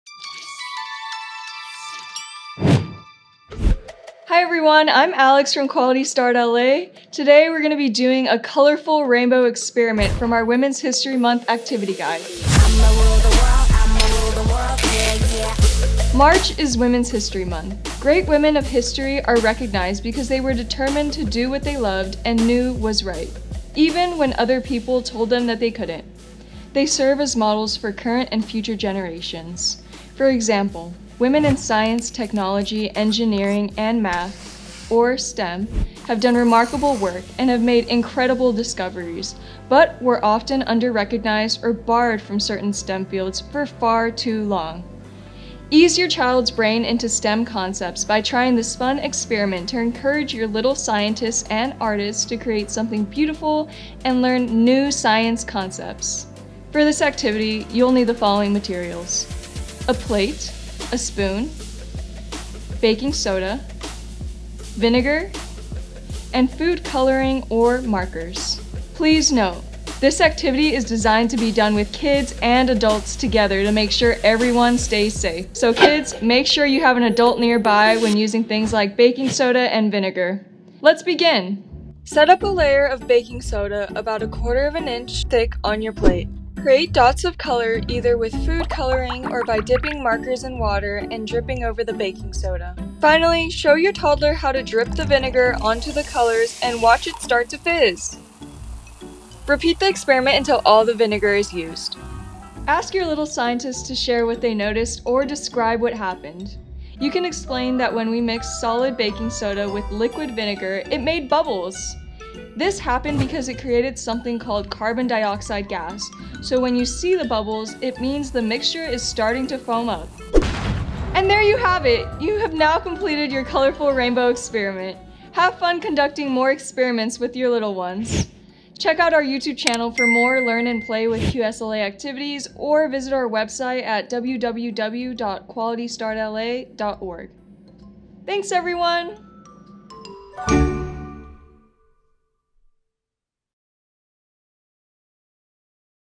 Colorful Rainbow Activity Celebrating Women's History Month | Learn and Play with QSLA
Support your little one's interest in science with this colorful rainbow experiment! Join our QSLA teammates as they guide you through this fun learning activity

To learn more about QSLA and explore our resources, visit our

*Please ensure proper supervision when children under 5 are using things like vinegar and baking soda!